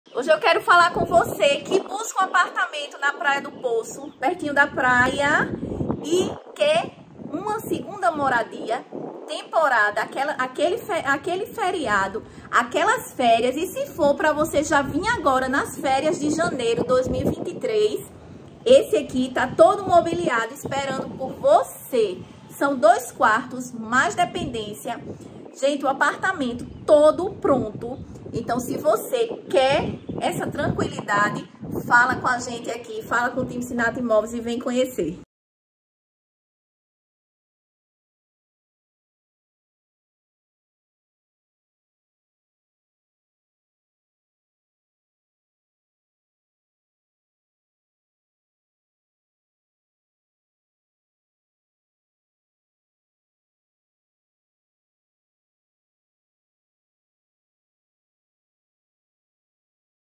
Indisponível🚫apto mobiliado no Poço por R$ 360 Mil com 2 quartos e vista mar em Cabedelo-PB
►►►É só chegar com as malas e apreciar a vista para o mar!
 (Cod.5103)

►Assista ao vídeo completo no canal do Youtube!

Você irá morar neste apartamento todo mobiliado, no quinto andar, com dois quartos, sendo uma suíte, com sala, cozinha, varanda, área de serviço, móveis planejados, DCE, vista para o mar, elevador e uma vaga de garagem.

►Valor: R$ 360 Mil
►Condomínio: R$ 953,00
►IPTU (anual): R$ 360,00
►Metragem: 95,79 m²
►Posição: Nascente Norte
►Localização: Rua Carolino Cardoso, no Bairro Poço em Cabedelo - PB